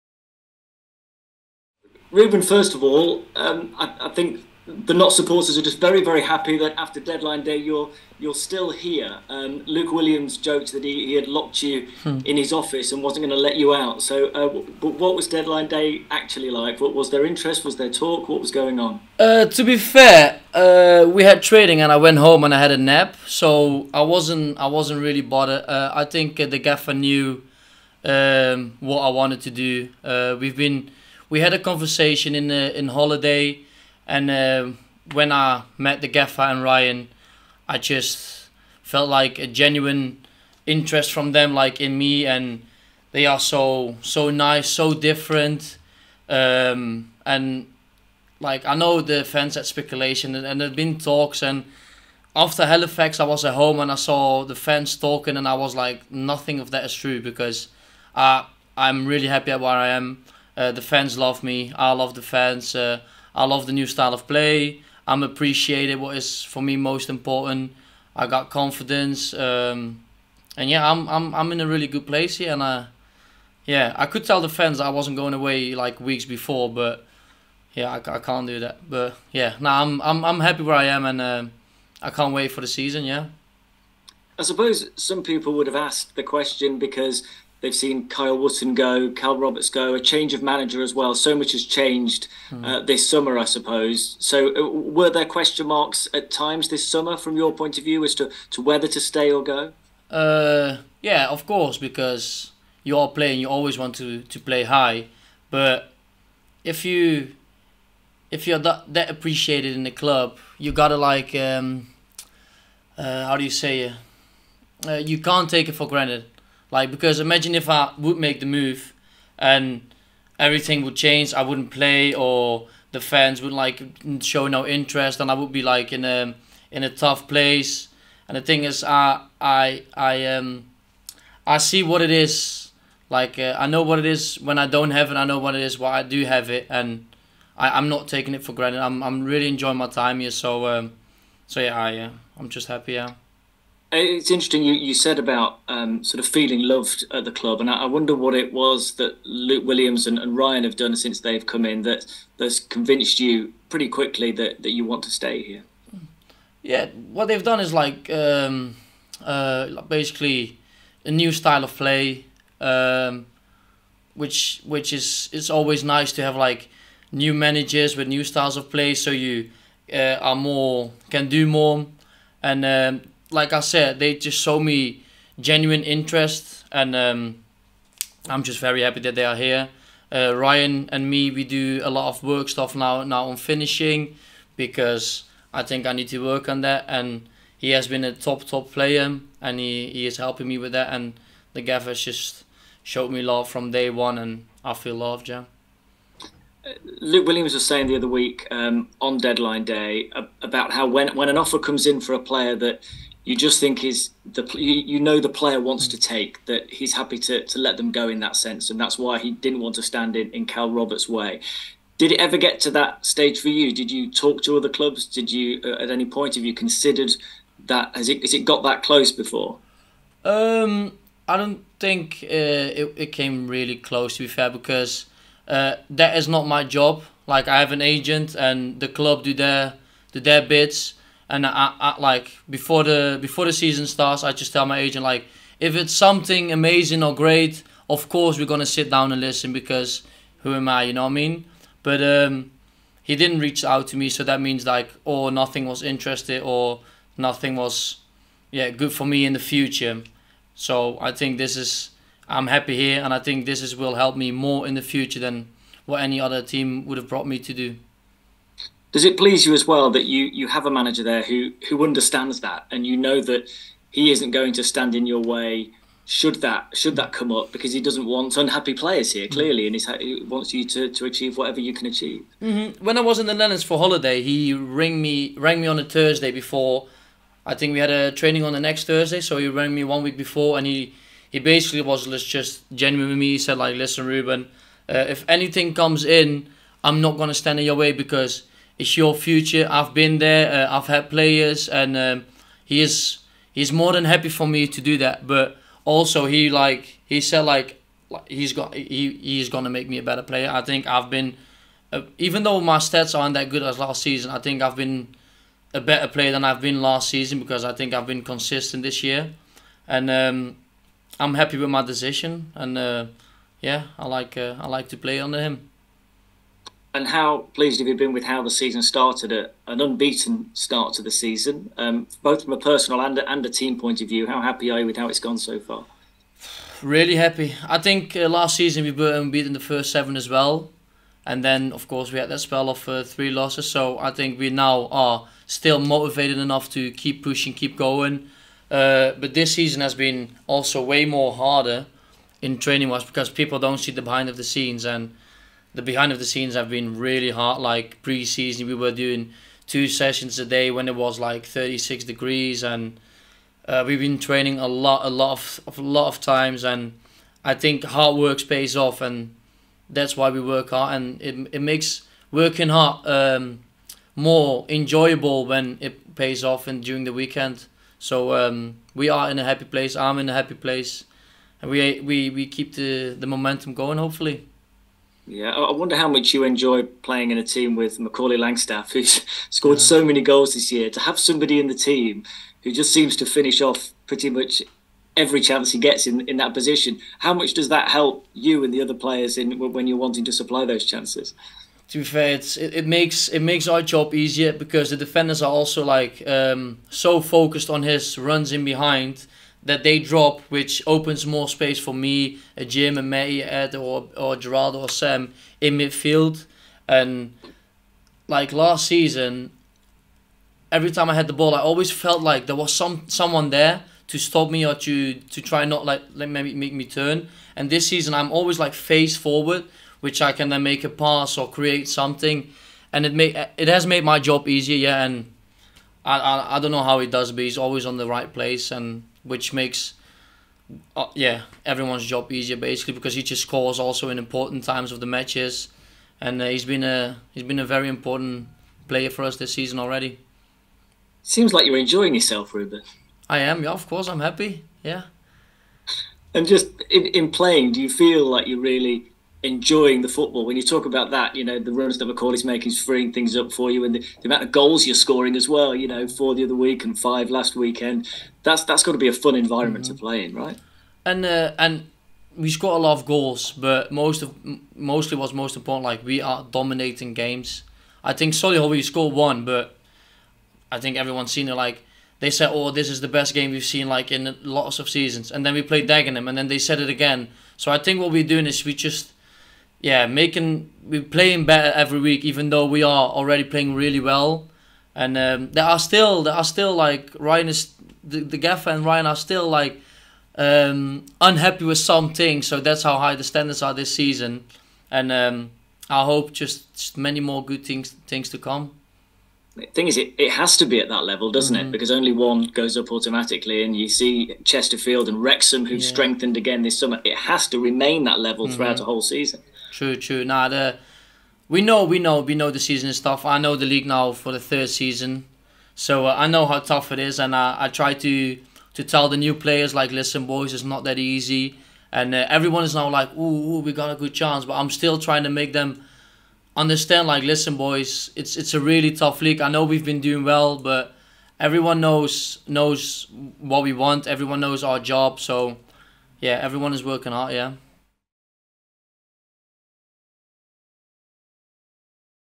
PRESS CONFERENCE | BROMLEY (H) | RUBEN RODRIGUES
Ruben revealed how much he's enjoying life at Meadow Lane ahead of the Ravens' visit.